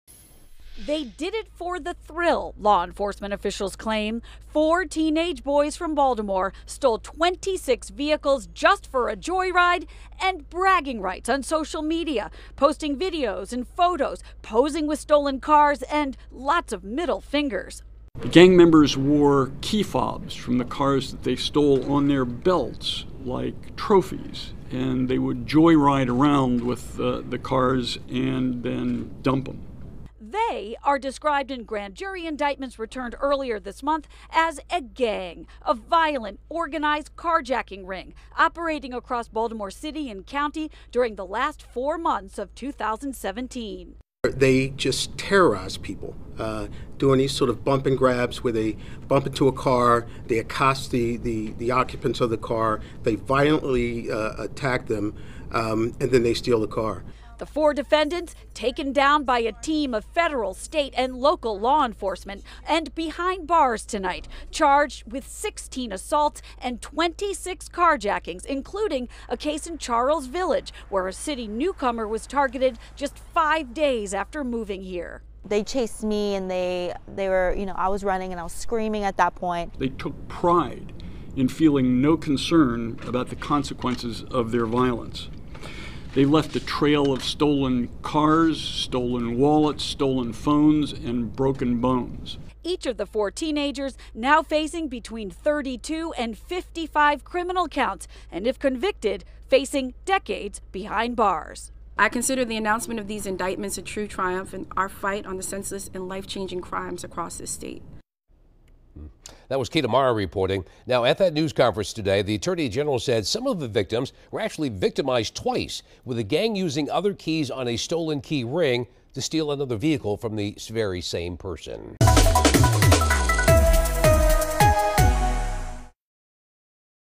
Teens arrested for 26 carjackings, officials say
Law enforcement officials announced Monday that they have squashed a violent carjacking gang that had been operating in Baltimore City and county. Four teenage suspects that were caught when local, state and federal law enforcement teamed up to take them down.